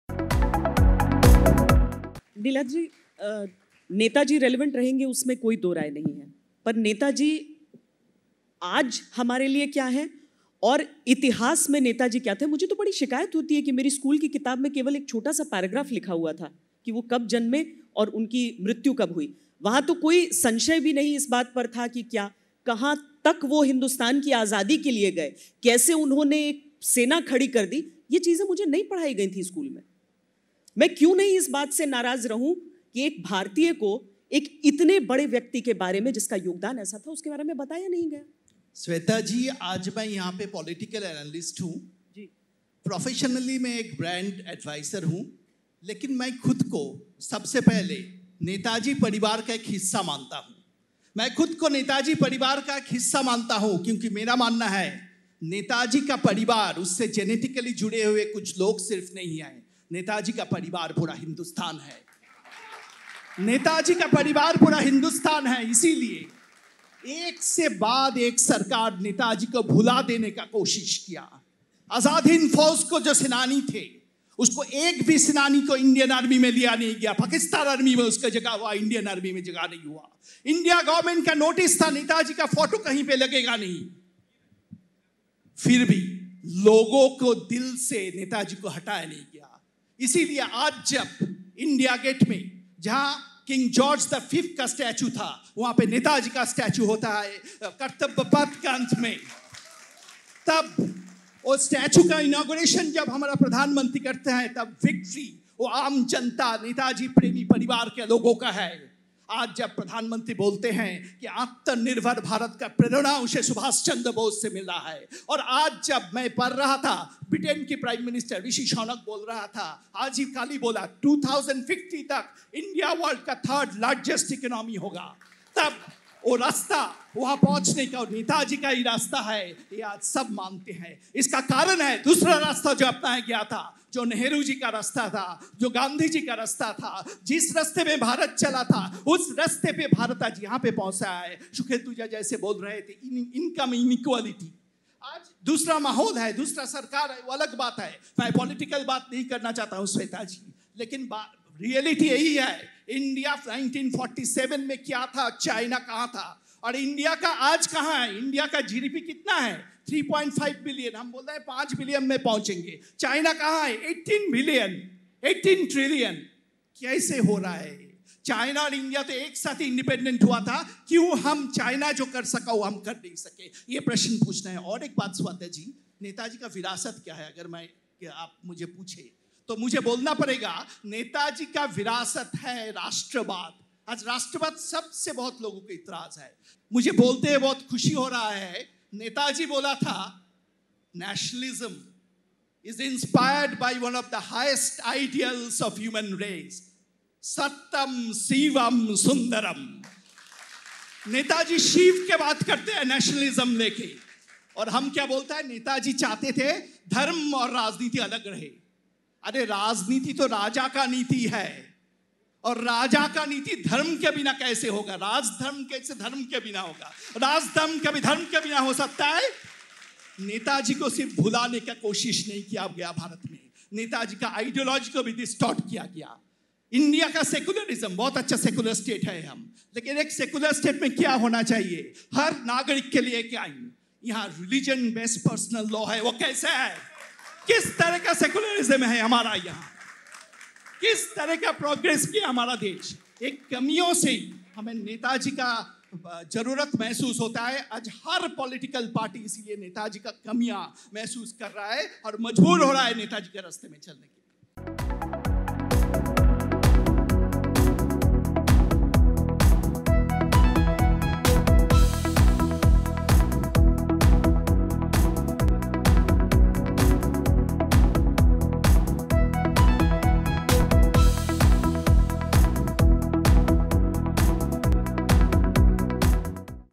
Subhash Chandra Bose | एक के बाद एक सरकार ने की है Netaji को भुला देने की कोशिश - Niladri Banerjee
एक के बाद एक सरकार ने की है नेताजी को भुला देने की कोशिश... नीलाद्रि बैनर्जी ने नेताजी सुभाषचंद्र को लेकर क्या कहा? सुनें सिर्फ़ साहित्य तक पर.